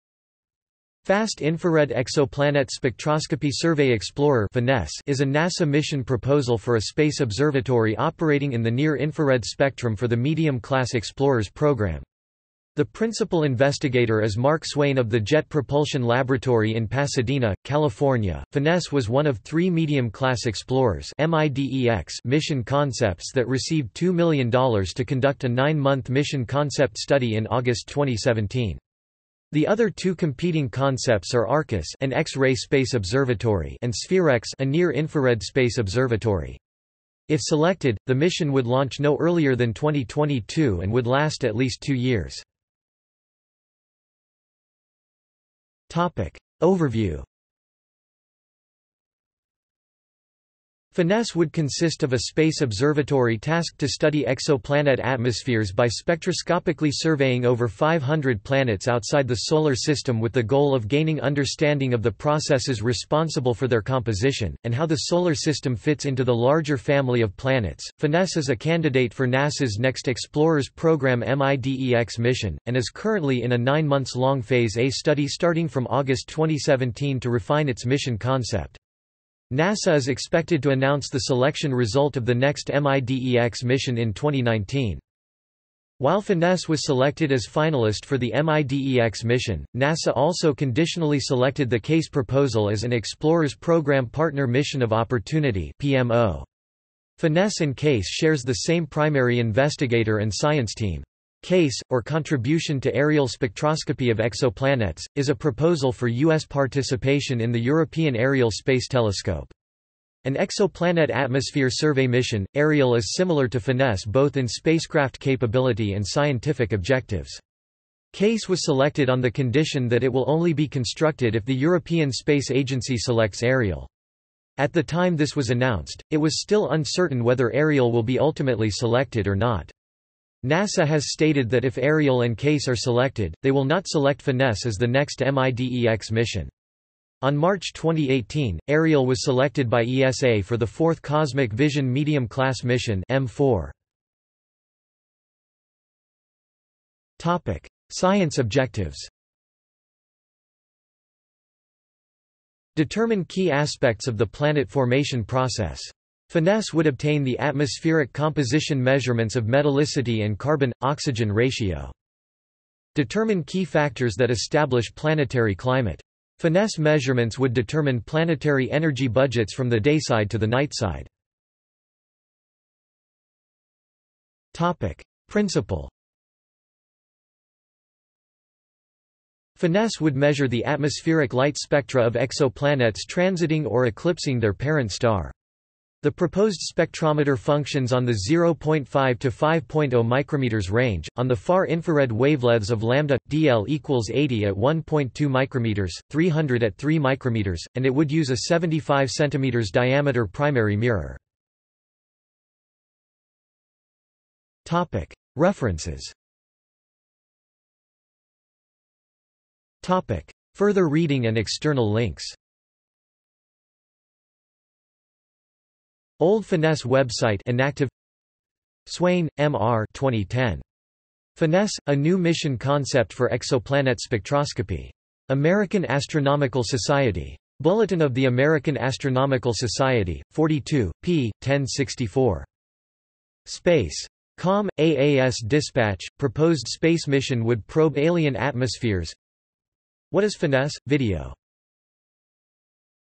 Fast Infrared Exoplanet Spectroscopy Survey Explorer | Wikipedia audio article
This is an audio version of the Wikipedia Article:
Fast Infrared Exoplanet Spectroscopy Survey Explorer

- Socrates

SUMMARY
Fast Infrared Exoplanet Spectroscopy Survey Explorer (FINESSE) is a  NASA mission proposal for a space observatory operating in the Near-infrared spectrum for the Medium-Class Explorers program. The Principal Investigator is Mark Swain of the Jet Propulsion Laboratory in Pasadena, California.FINESSE was one of three Medium-Class Explorers (MIDEX) mission concepts that received $2 million to conduct a nine-month mission concept study in August 2017. The other two competing concepts are Arcus (an X-ray space observatory) and SPHEREx (a near-infrared space observatory). If selected, the mission would launch no earlier than 2022 and would last at least two years.